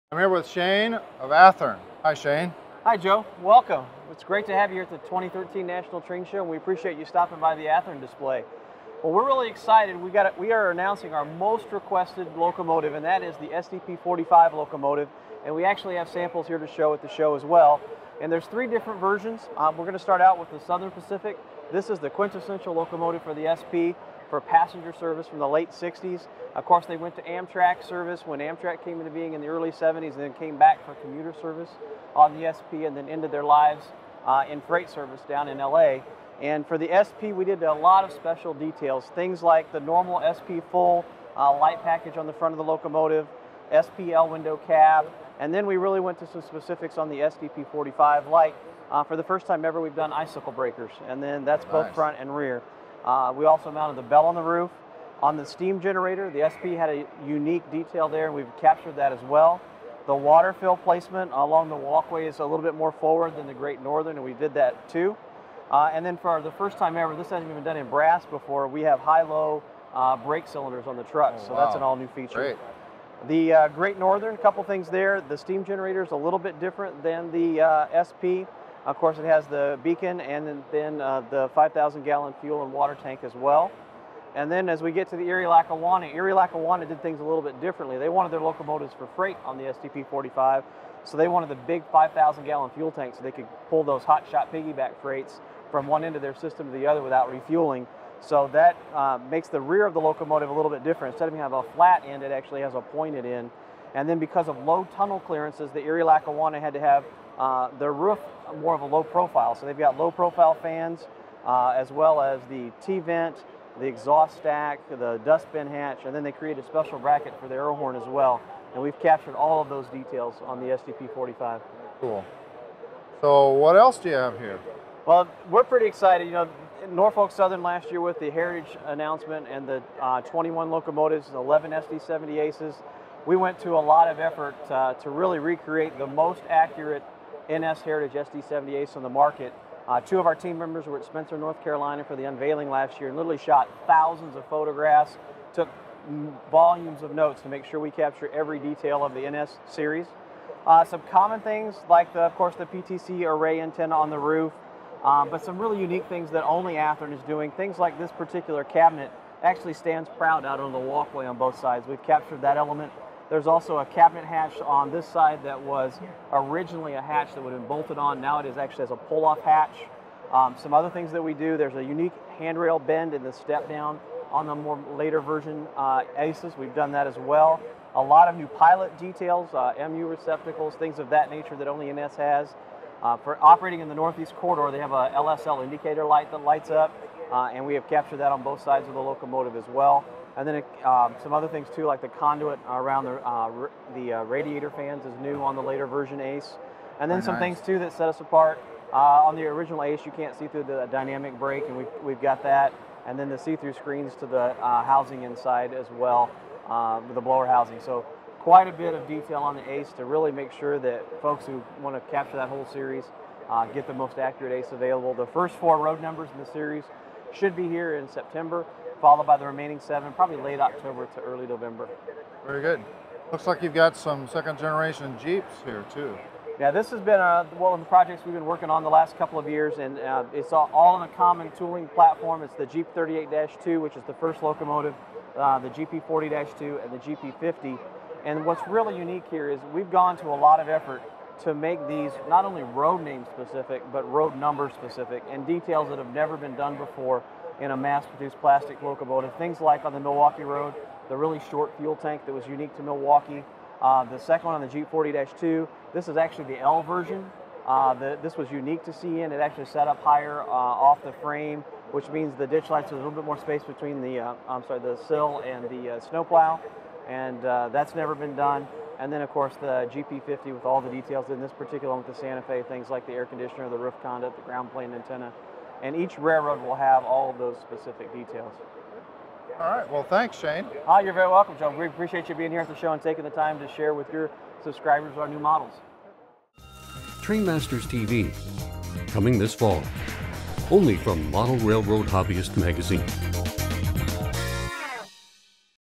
Athearn National Train Show Atlanta | July 2013 | Model Railroad Hobbyist | TrainMasters TV
- Model Railroad Hobbyist magazine visits with Shane Wilson of Athearn at the National Train Show in Atlanta, GA, July 2013. See their new SDP45, Heritage units, and new GP diesels in HO.